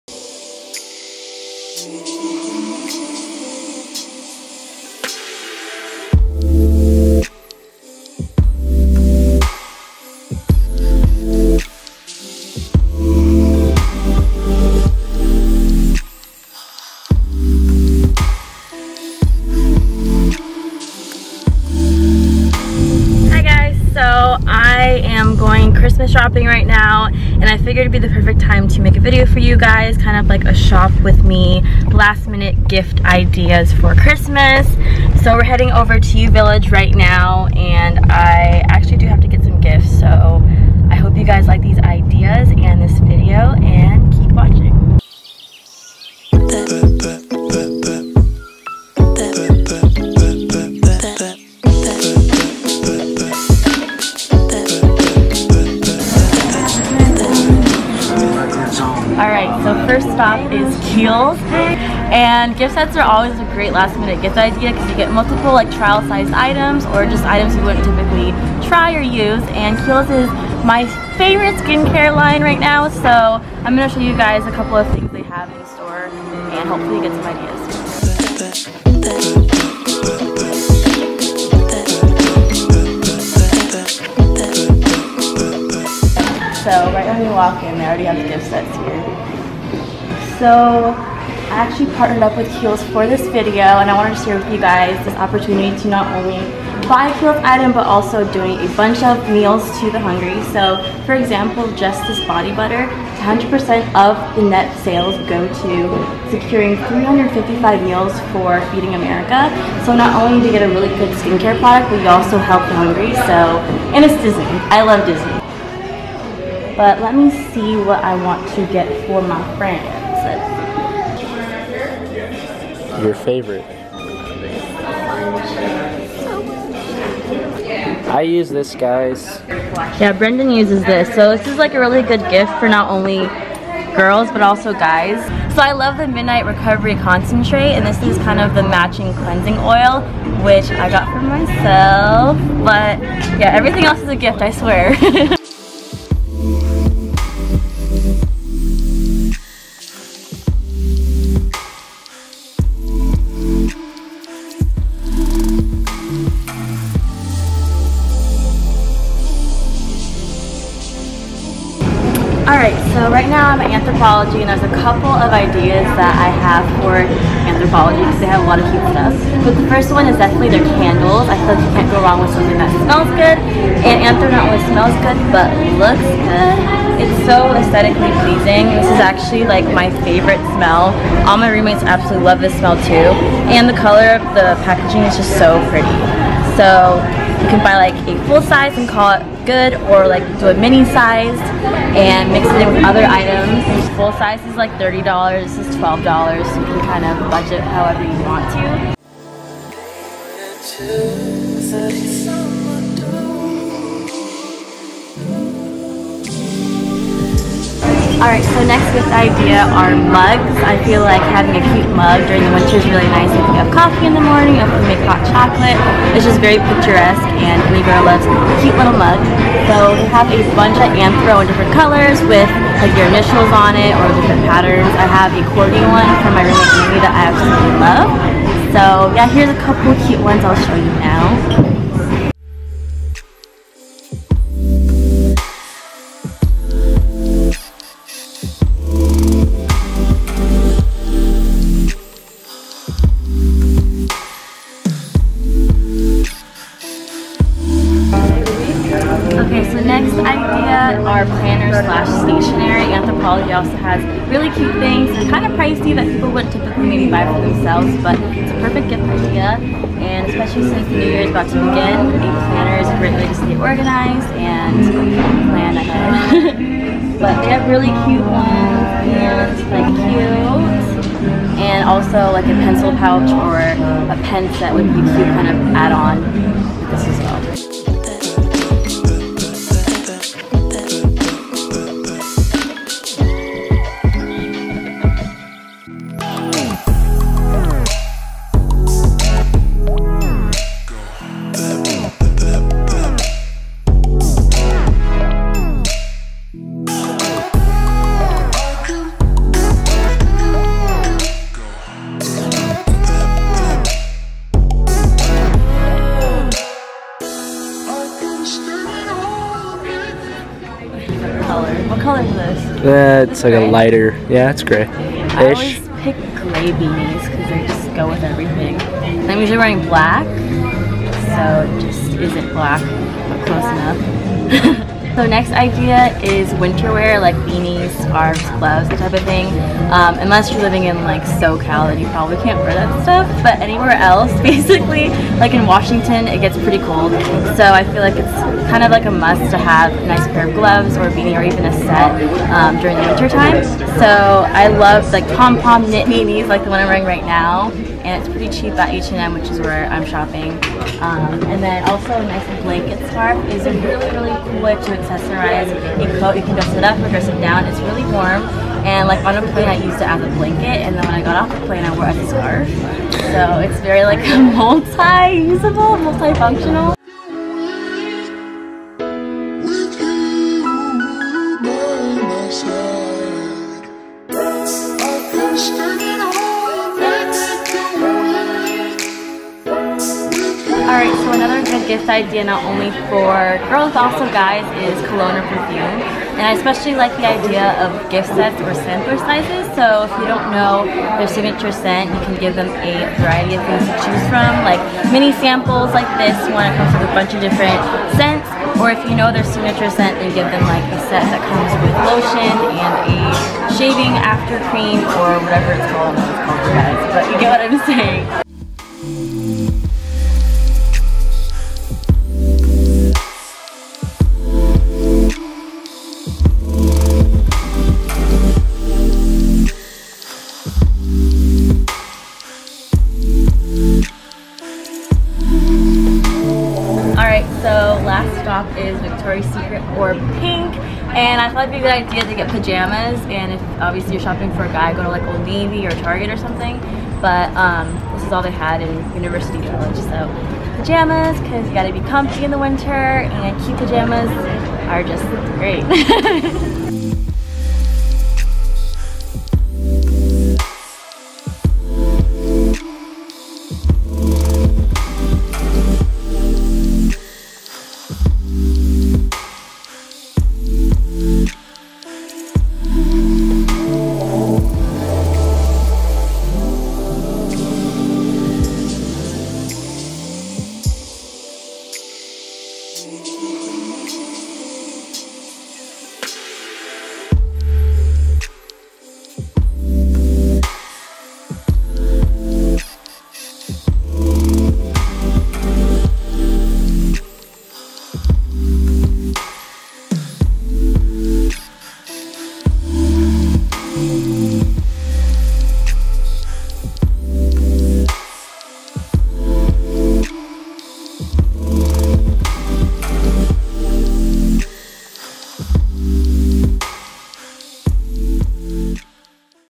SHOP WITH ME - last minute gift ideas | viviannnv
Hi loves! For all you procrastinators - I made something especially for you lol

follow jasper!! IG: @jasper_corgi

Get a Free Uber Ride with code: "vivianv42"